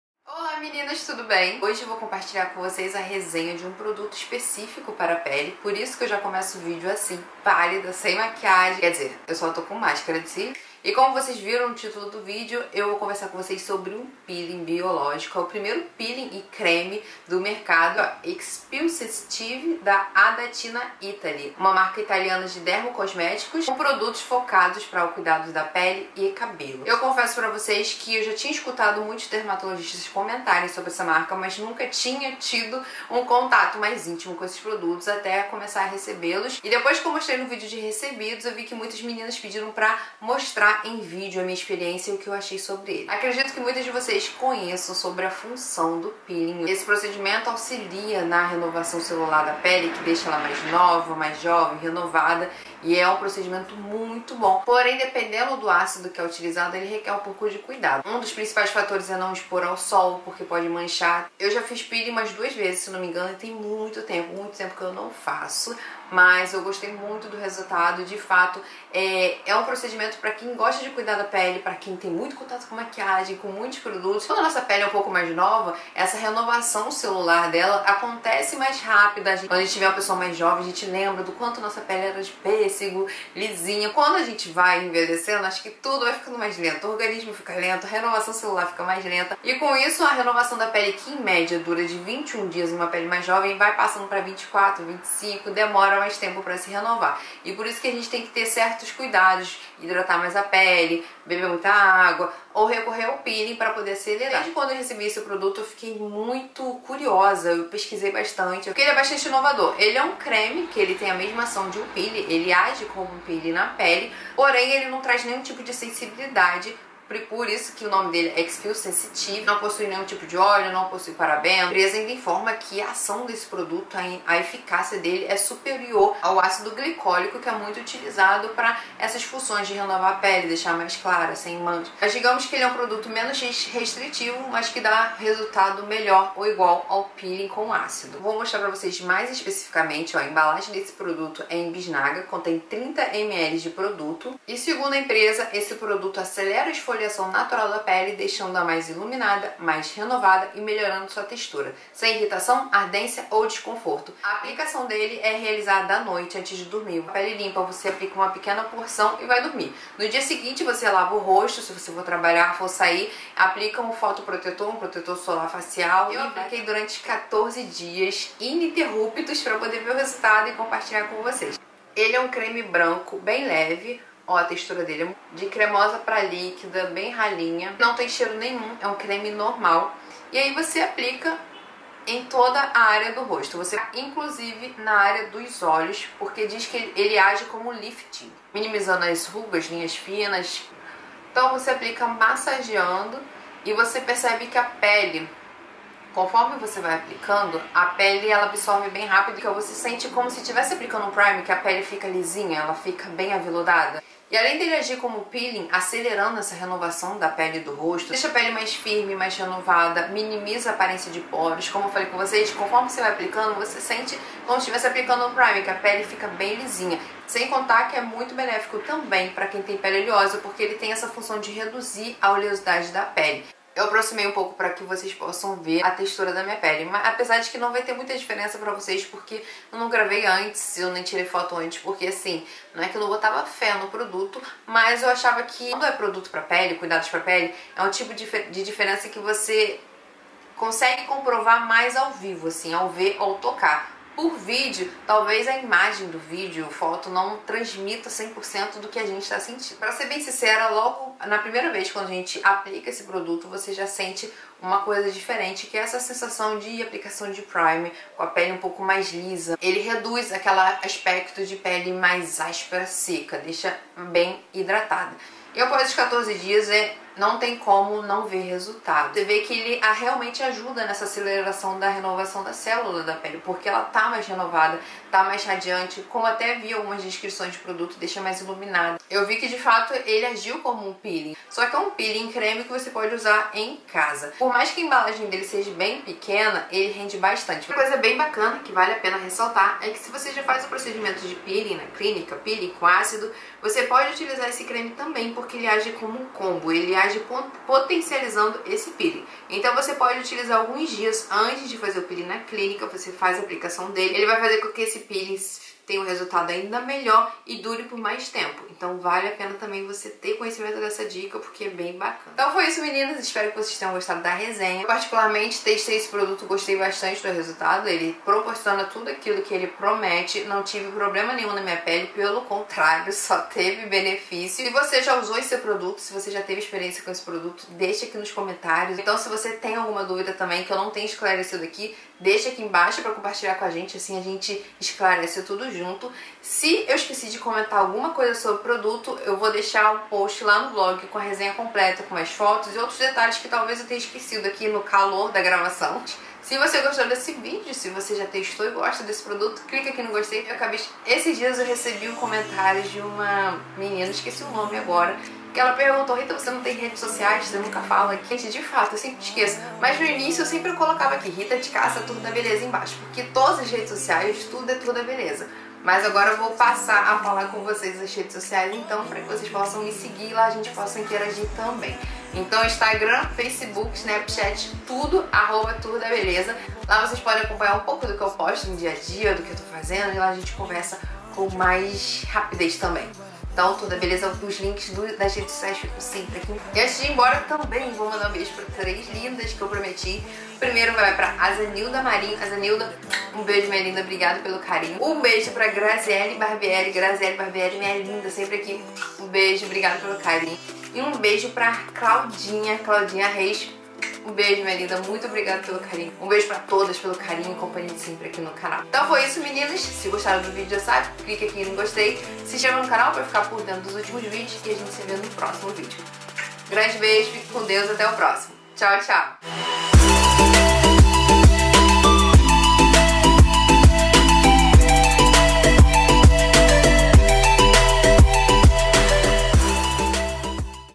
PEELING EM CREME X-PELL SENSITIVE ADA TINA
Vocês encontram esse produto em algumas drogarias e lojas virtuais! Pesquisei pela internet e encontrei em algumas lojinhas: